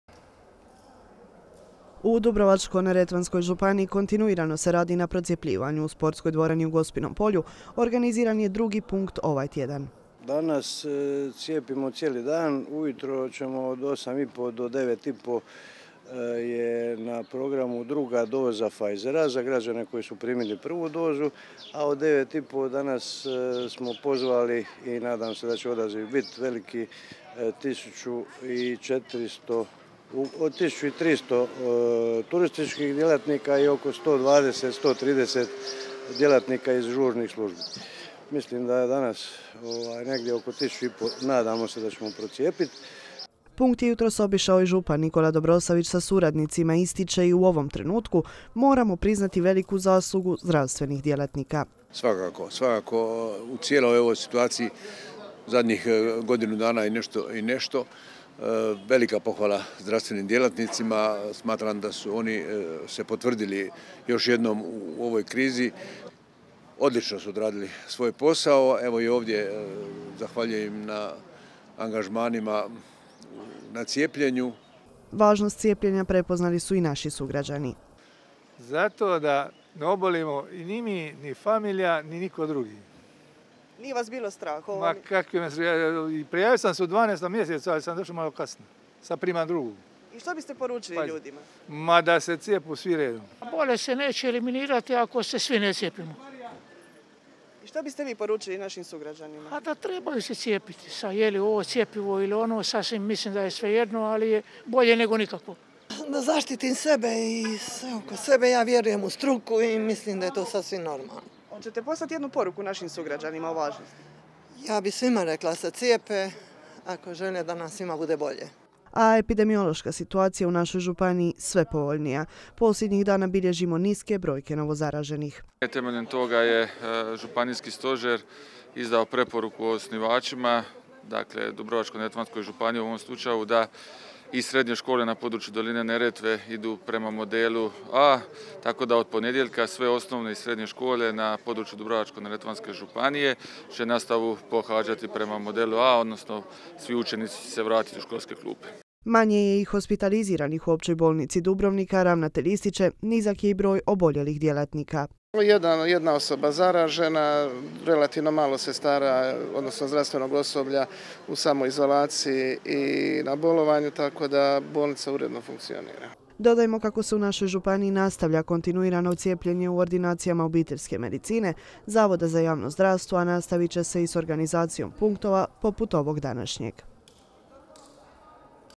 Novi punkt organiziranog cijepljenja u Gospinom polju
Oko 1500 naših sugrađana danas je cijepljeno na punktu u Gospinom polju. Punkt je jutros obišao i župan Nikola Dobroslavić.